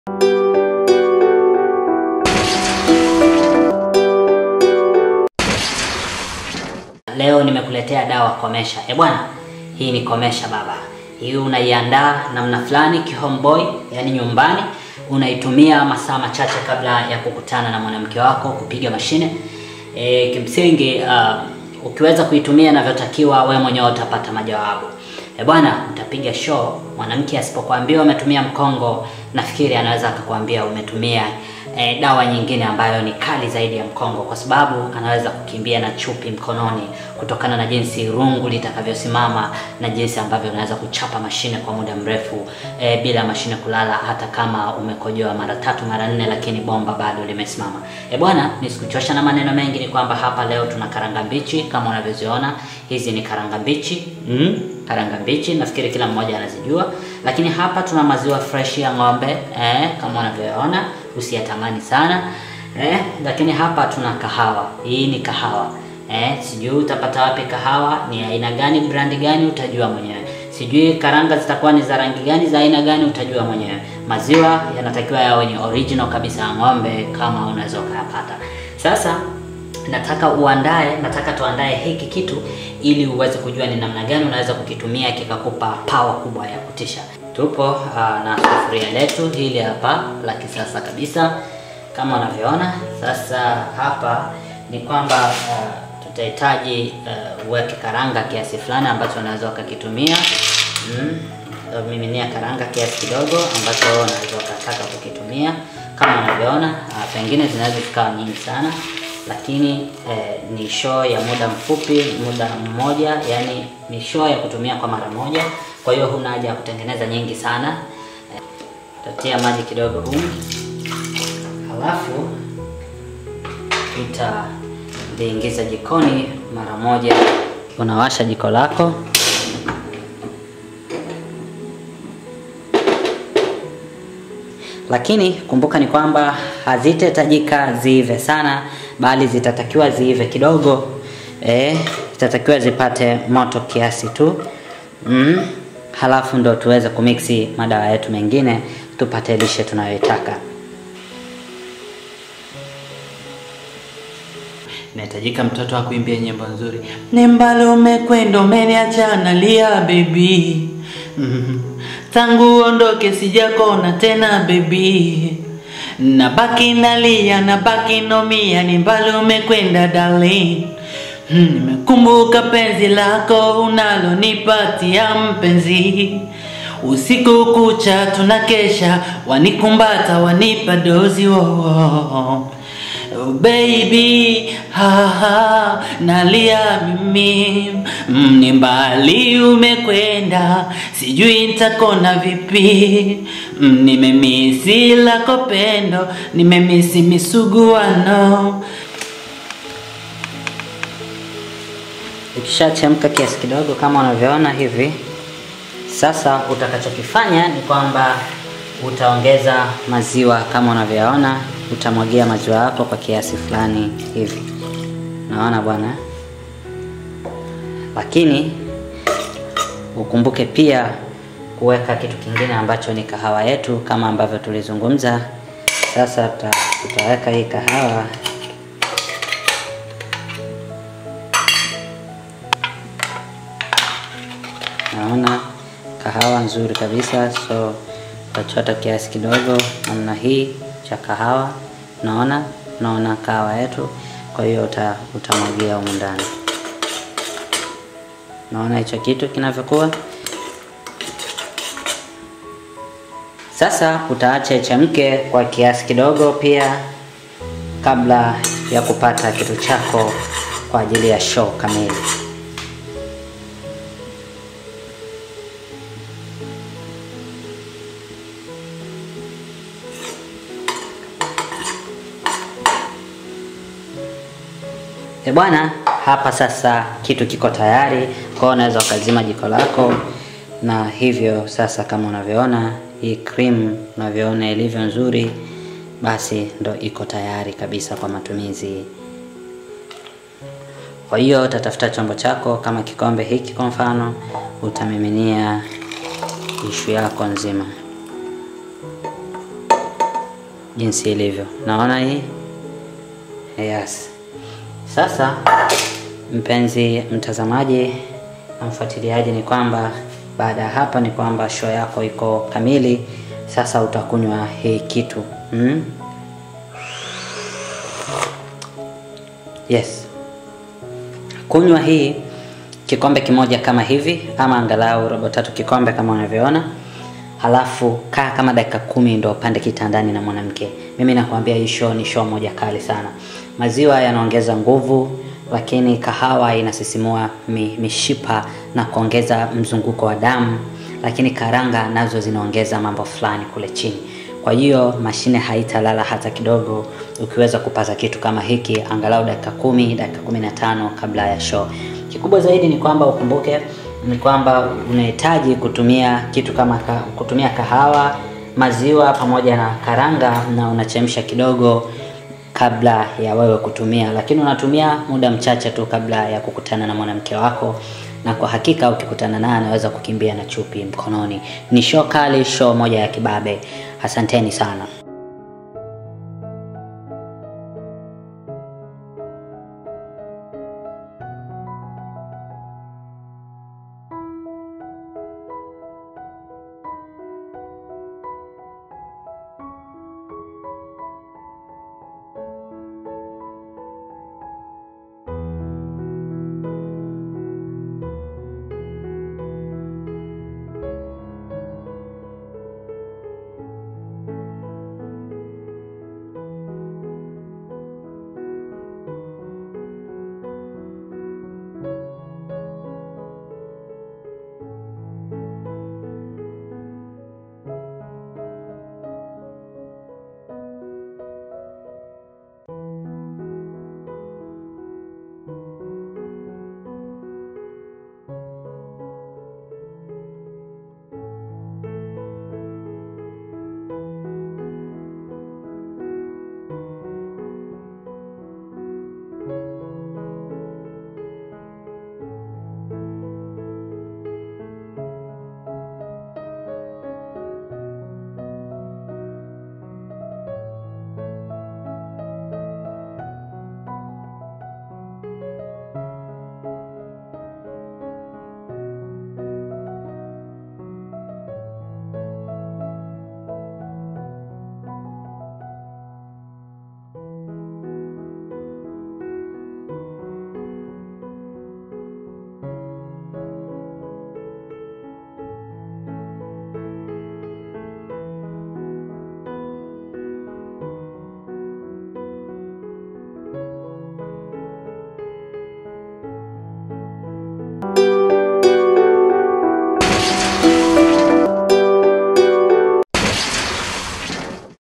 Dawa Nzuri ya kurudisha heshima ya mwanaume/ KIBOKO YAO
Tumia hii dawa kama una mtu, kisha itumie na utupe mrejesho. Hii ni dawa kiboko ya kurejesha amani kwenye ndoa za watu.